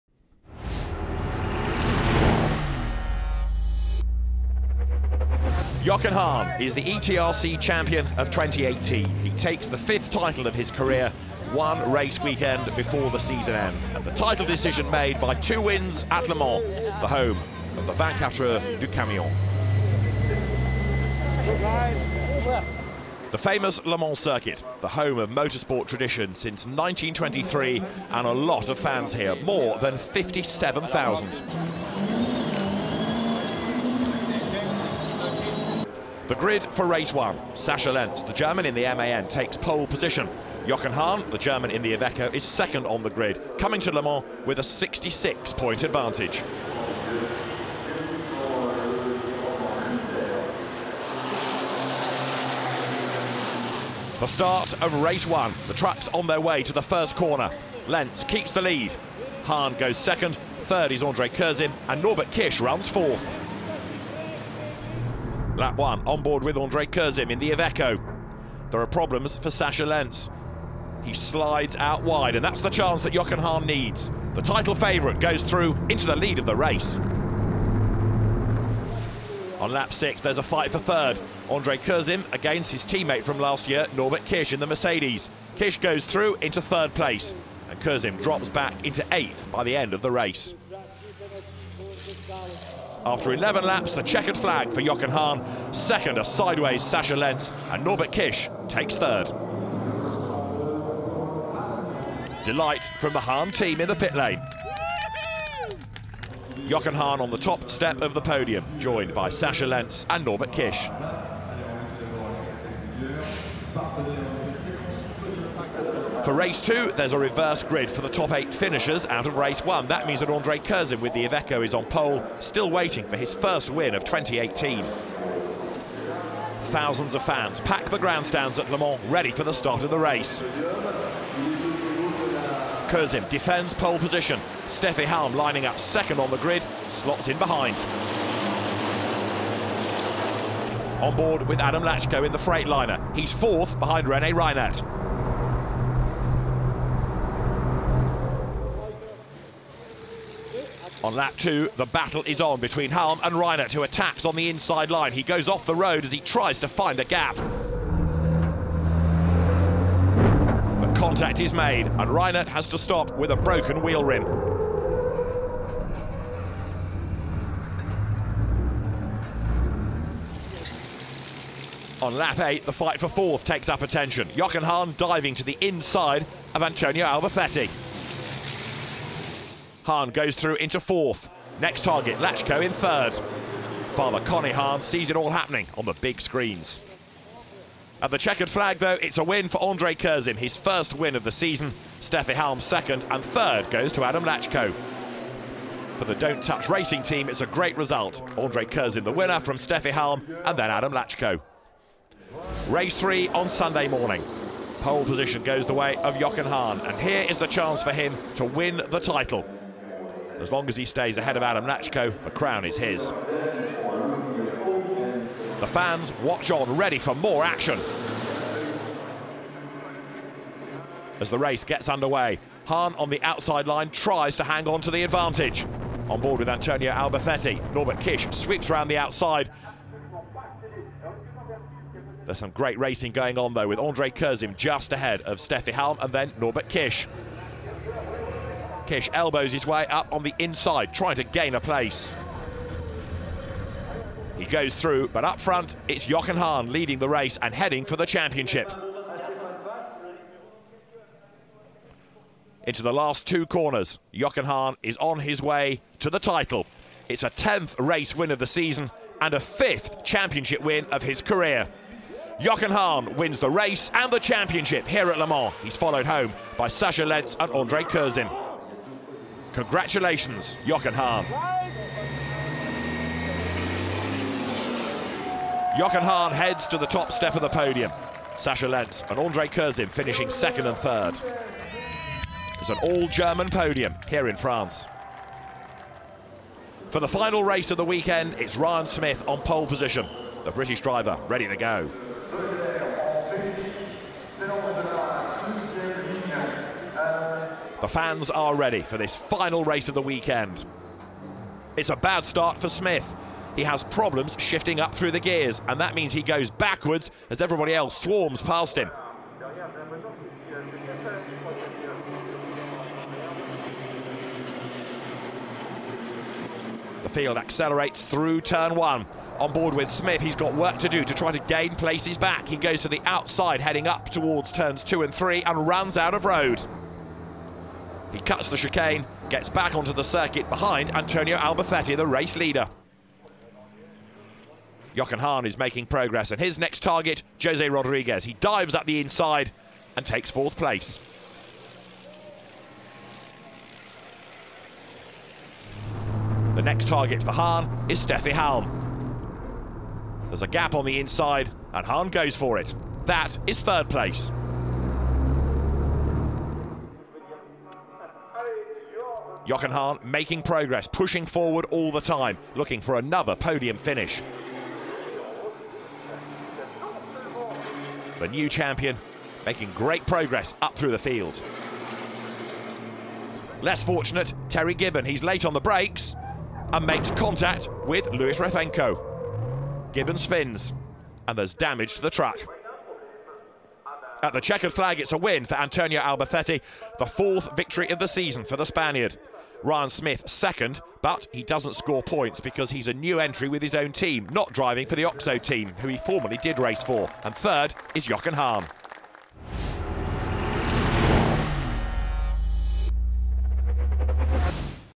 FIA ETRC LeMans 2018 - News Edit
Review all the action from Le Mans 2018. One more stage to go for the 2018 FIAETRC.
Next stop: Jarama/ESP - October 6/7